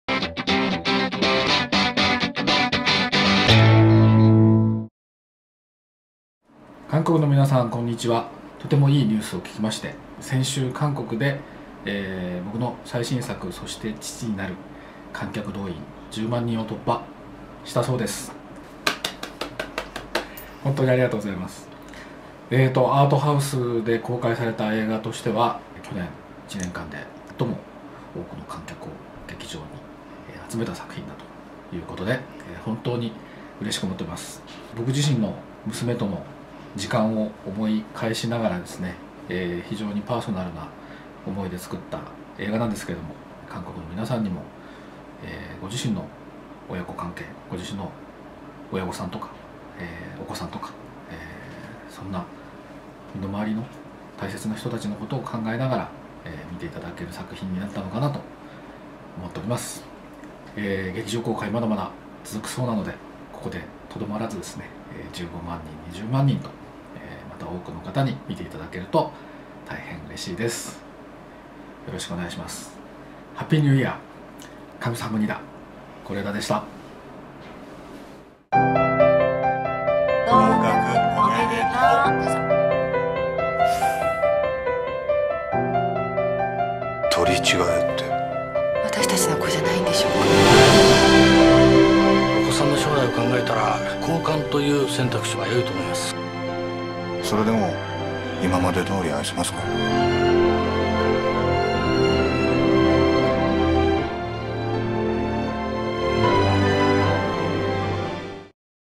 [AJU TV] '그렇게 아버지가 된다' 감독, 한국 관객 반응에 깜놀
지난 12월 19일 개봉하여 개봉 3일 만에 1만 관객을 돌파한데 이어 14일 만에 5만 관객, 39일 만에 10만 관객을 돌파. 그리고 46일 만에 11만 고지를 점령한 올 겨울 최고의 화제작 '그렇게 아버지가 된다'가 고레에다히로카즈 감독이 한국 관객에게 직접 전하는 감사 메시지 영상을 공개했습니다.

영화 '그렇게 아버지가 된다'는 현대 일본영화를 대표하는 거장 고레에다히로카즈의 섬세하고 빼어난 연출과 일본의 톱스타 후쿠야마마사하루의 '아버지'로의 연기 변신, 보석 같은 아역 배우들의 놀라운 앙상블로 2013 칸영화제 심사위원상을 수상하고 세계적 거장 스티븐스필버그를 비롯한 전세계 언론과 평단의 만장일치의 극찬세례를 받은 영화입니다.

개봉 39일 만에 10만 관객을 돌파하는 진기록을 세우며 오래도록 잊지 못할 감동과 여운으로 관객들의 마음을 사로잡은 올 겨울 최고의 화제작 '그렇게 아버지가 된다'는 현재 절찬 상영중입니다.